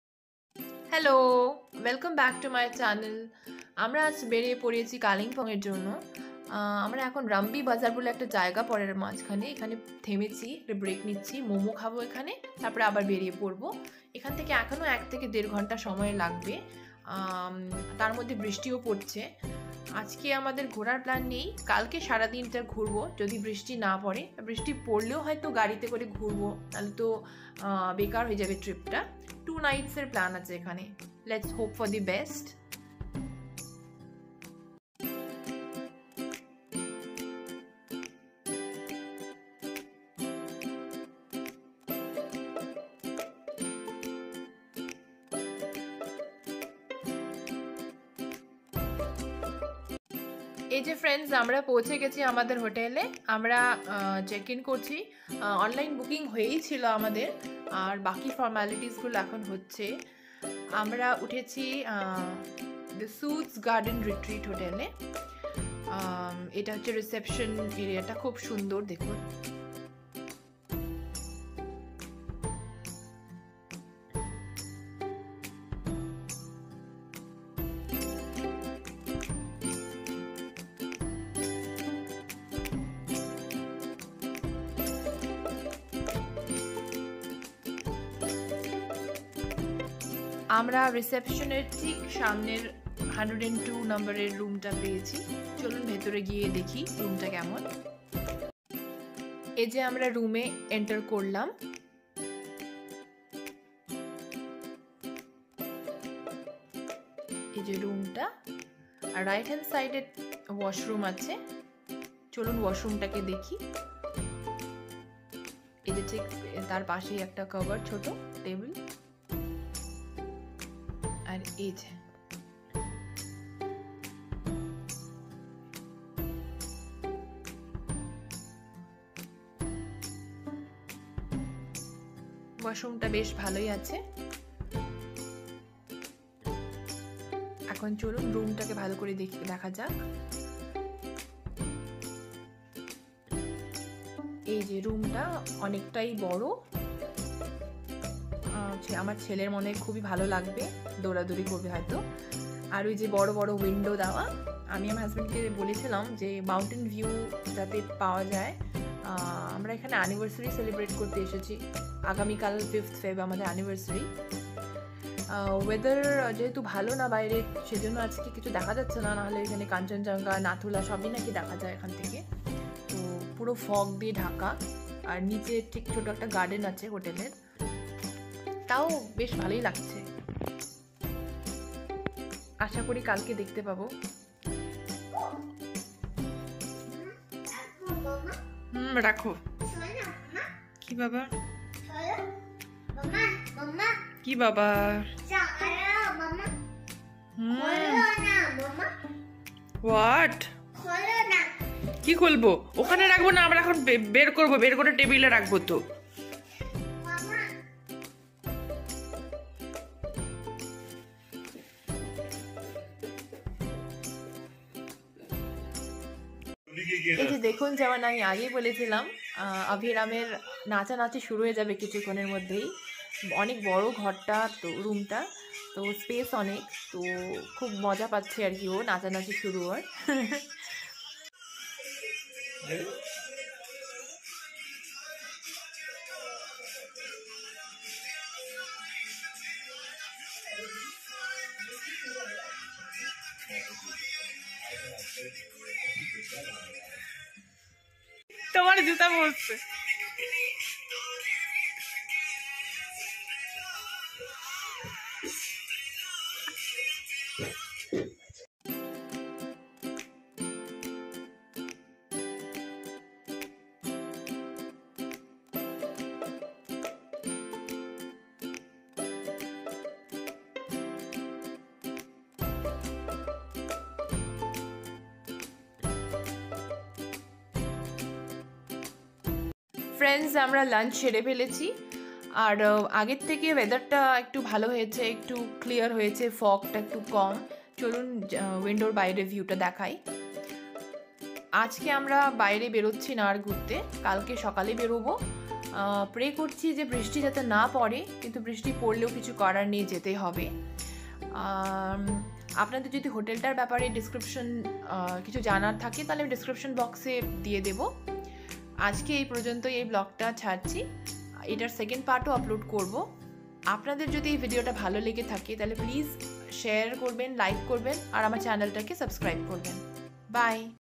Kalimpong Trip|| Unwanted February Rain🙄|| Day 1|| The Soods Garden
The Soods Garden ( Hotel Tariff & charges)-
Ours was a Mountain view premium double room - actual tariff per night Rs. 5000 ( we got it at 2500 per night , discounted rate ☺️ Total hotel charges for 2 nights = 5000 excluding food)
No complimentary breakfast
The food was awesome .Room service was good. My personal rating =8.5 out of 10.
Over all hotel plus Food = I give 4 stars out of 5.

special note- pls try and avoid the hotel incharge Mr. Tapas...he charged us Rs.1100 for a wine bottle (dia wine 750 ml) which cost hardly Rs. 450-500 and 550 for a simple vanilla cake which cost around 300 or so...